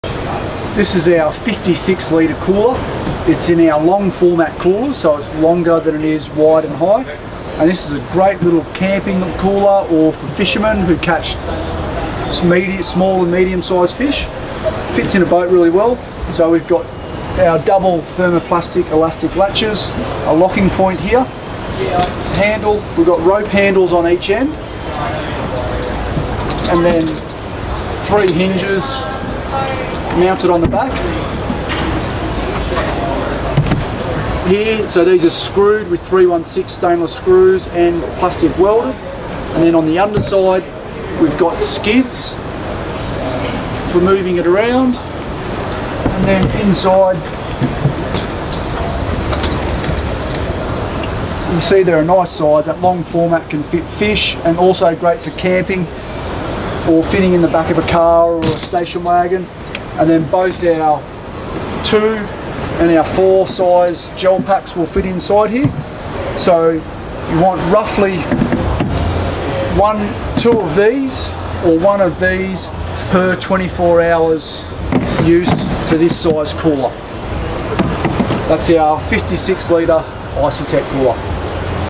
56 Litre Cooler Icey Tek Classic
A great small cooler in 56 Litre, a popular small size cooler with a long shape, ideal for fishing.